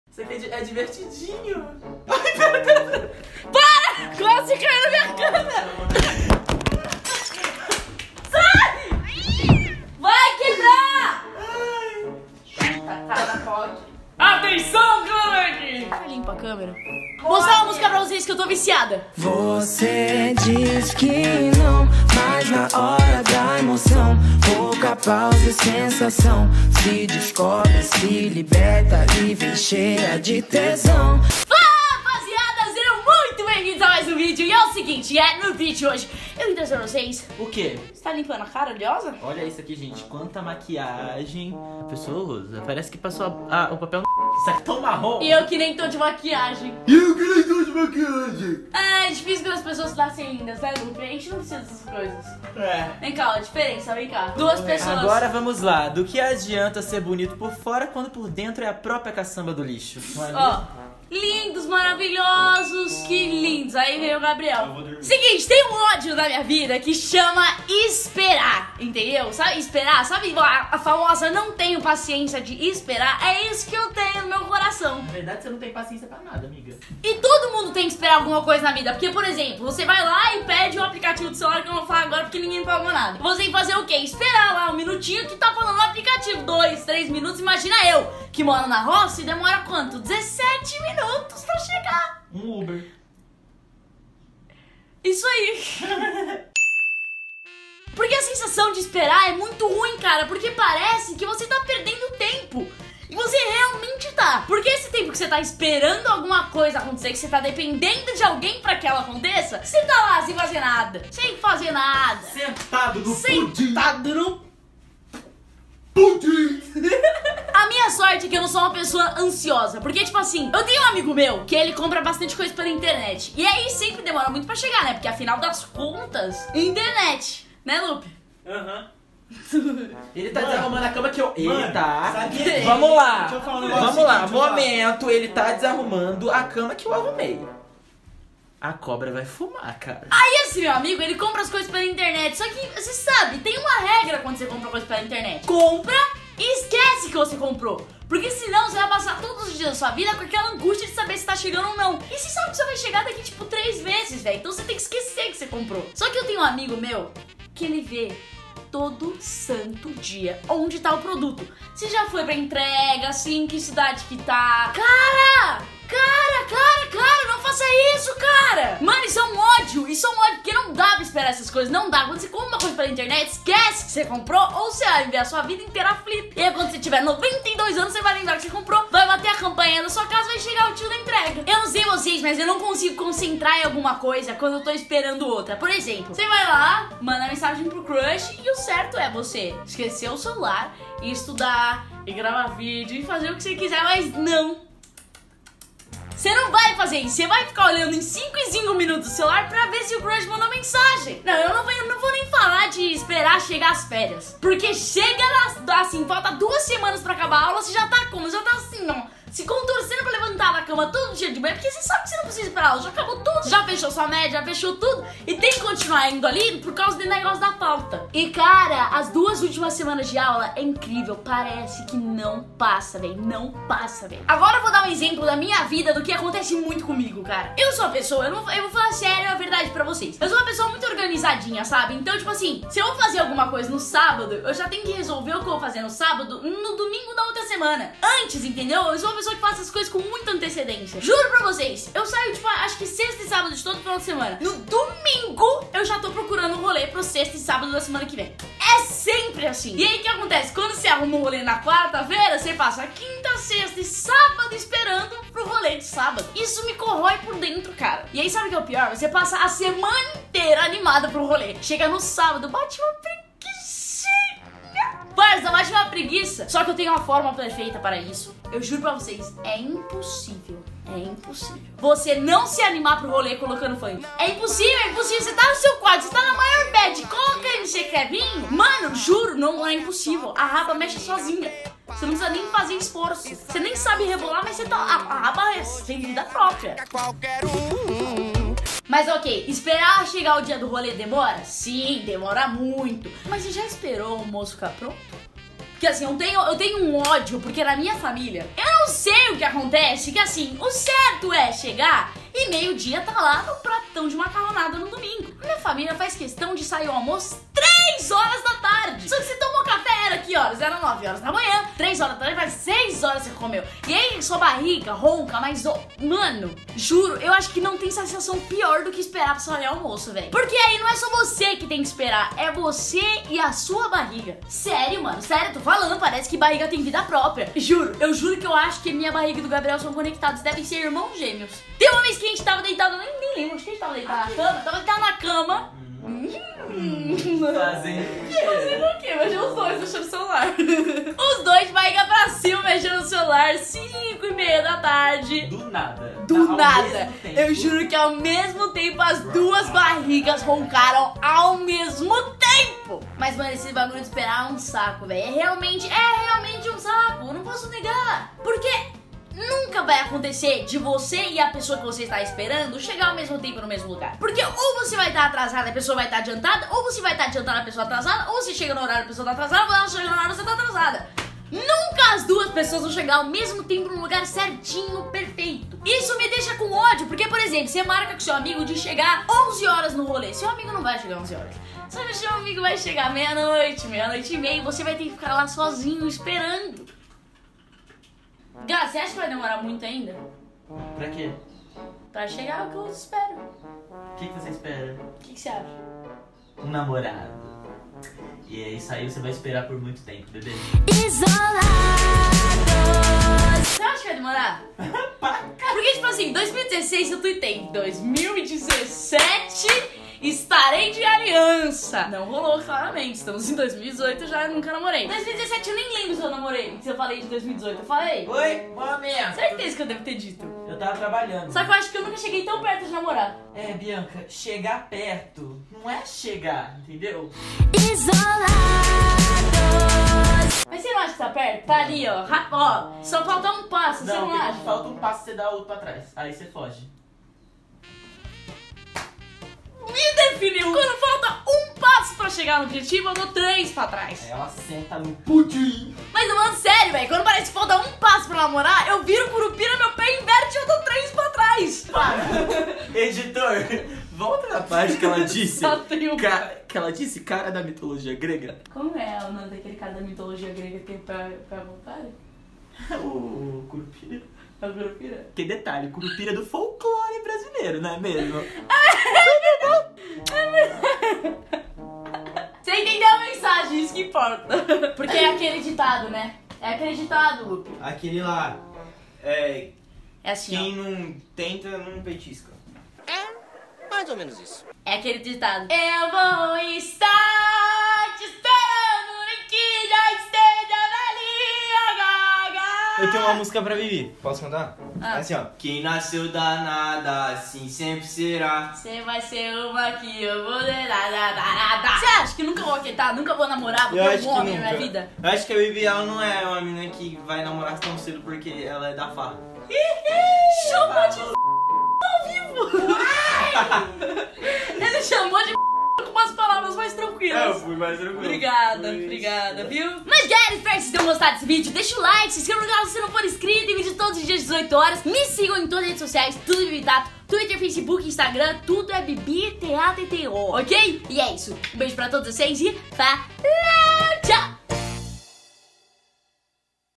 EU ODEIO ESPERAR
Nesse vlog Bibi desabafa o quanto ela acha irritante esperar!